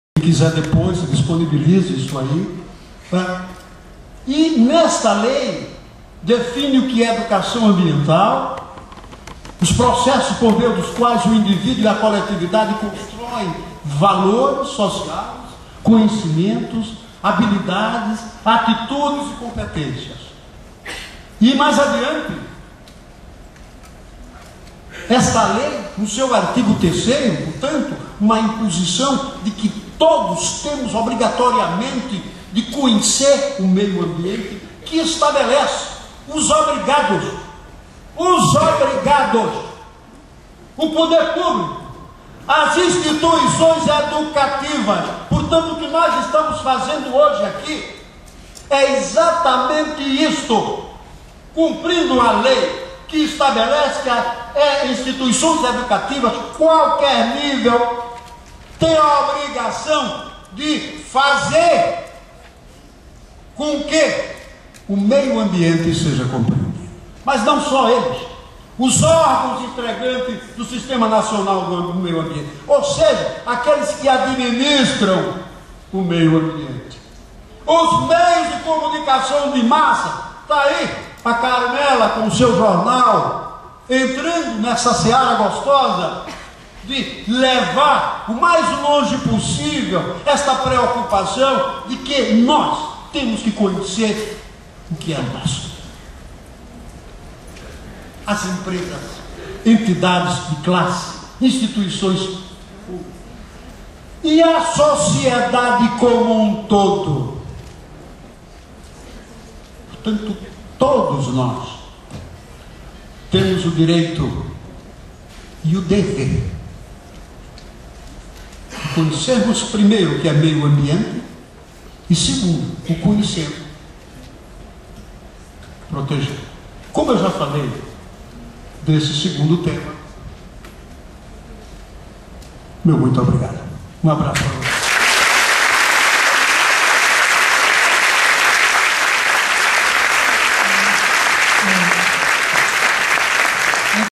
4 -Ciclo de Estudos Jurídicos - O Direito no lugar comum - Educação ambiental para a cidadania.
Wellington Pacheco Barros abrilhantou a noite dos estudantes de Direito da Universidade Feevale falando sobre a educação ambiental como Direito e dever de todos. Agradecemos a Feevale, aos estudantes e a todos os professores que contribuiram para a realização da 1ª edição do Ciclo de Estudos Jurídicos em Universidades. Vamos fomentar um novo olhar pelo Direito e esse trabalho começa por nós.